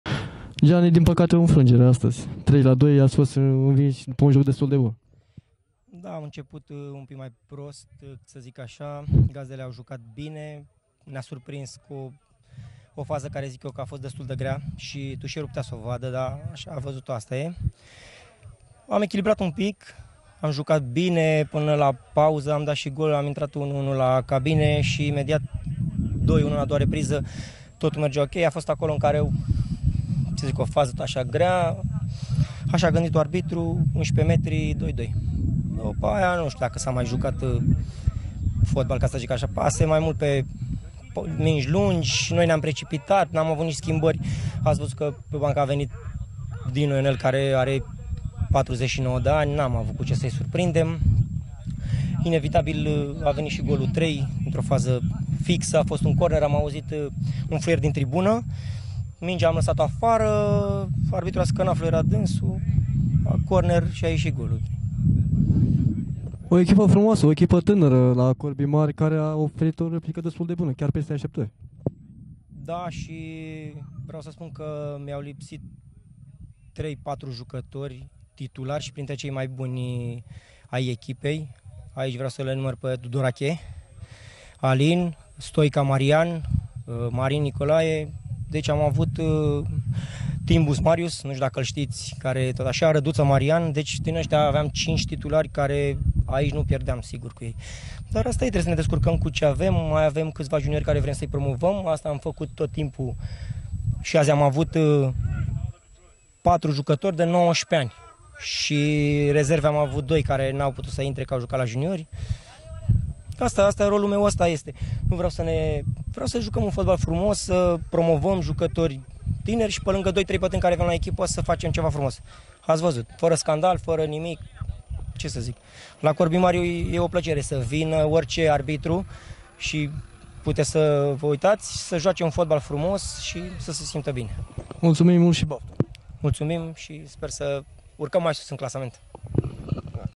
VIDEO. Interviu cu Giani Marin, capitanul echipei Corbii Mari
Corbii Mari a lasat o impresie frumoasa la Morteni, unde gazdele ai invins cu 3-2 dupa un meci interesant. Tanara echipa din Corbii Mari a lasat o impresie frumoasa, in ciuda infrnagerii suferite. Fotbaljudetean.info a stat de vorba la sfarsitul partidei cu Giani Marin, capitanul echipei din Corbii Mari. Reporter: Costinel Neagu.